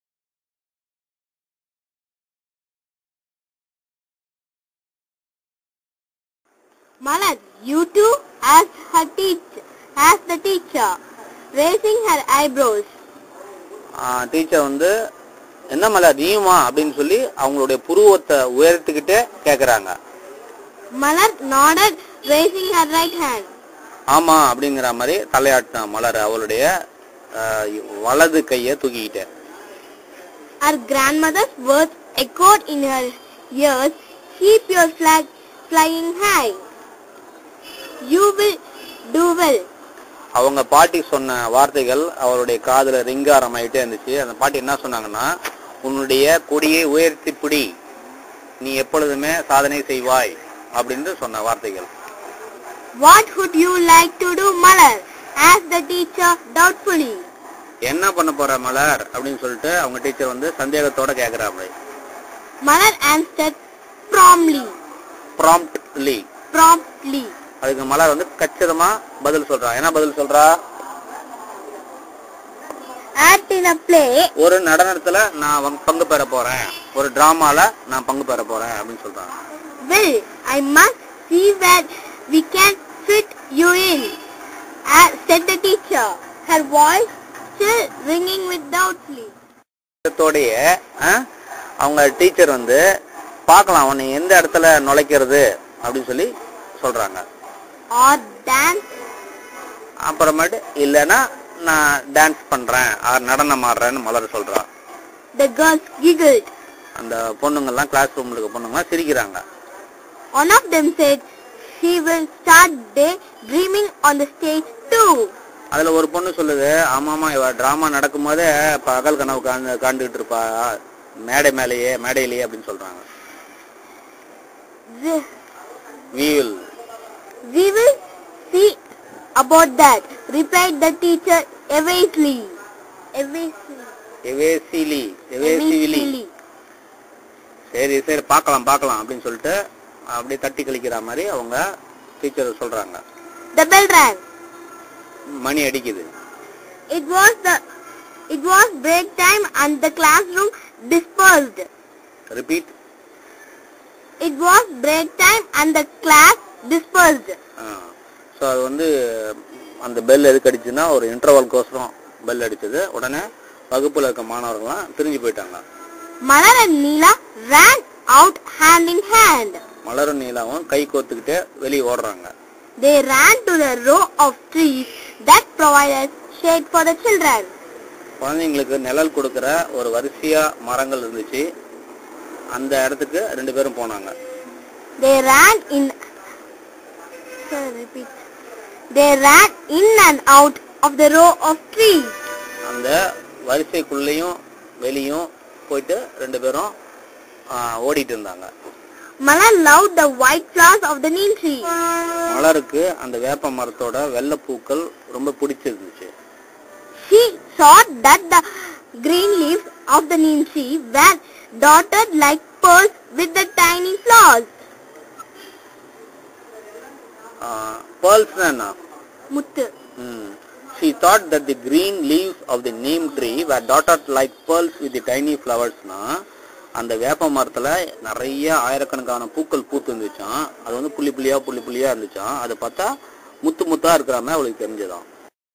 English lesson grade 6 NEEM Tree part 2 - video 228
A nice lesson on neem tree

 "Malar? You too..." asked the teacher, raising her eyebrows. Malar nodded, raising her right hand. Her grandmother's words echoed in her ears, "Keep your flag flying high. You will do well" .

"What would you like to do, Malar?" Malar said, "Act in a play".  "Well... I  must find out where you can fit you in." said the teacher, her voice still ringing with doubt. "Or...rr... dance."
The girls giggled. One of them said, "She will start day dreaming on the stage, too."
"We'll see about that," replied the teacher evasively. The bell rang.

It was break time and the class dispersed. Malar and Nila ran out hand in hand. They ran to the row of trees that provided shade for the children. They ran in and out of the row of the neem tree. She thought that the green leaves of the neem tree were dotted like pearls with the tiny flowers. Suddenly, Nila stopped running and pointed up to one of the trees.